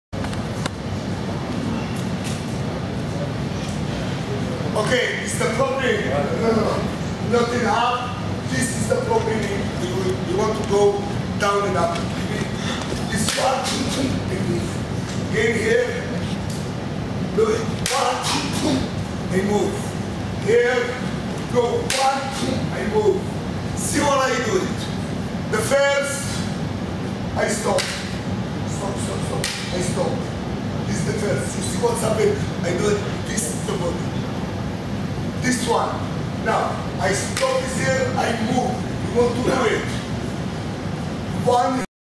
KRAV MAGA NYC - Gabi Noah and Tsahi Shemesh - Stick vs Stick
Gabi Noah demonstrates how to defend against a stick attack (when the defender has a stick).

Krav Maga NYC. Krav Maga Upper West Side. Self Defense NYC. Women Self Defense.